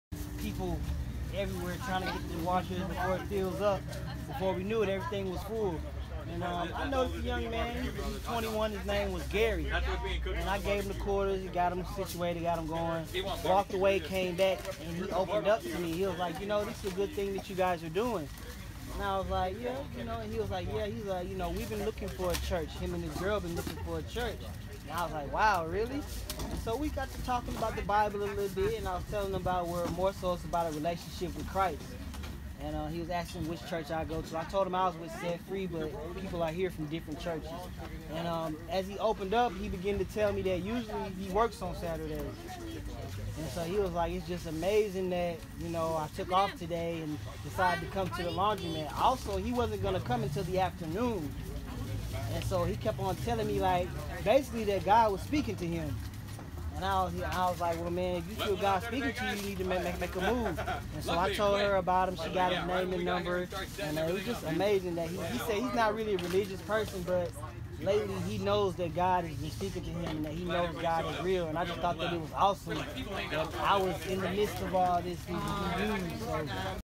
Willie's Laundry Love Story
Willie shares his experience of Laundry Love at Dream Center LE's City Serve '18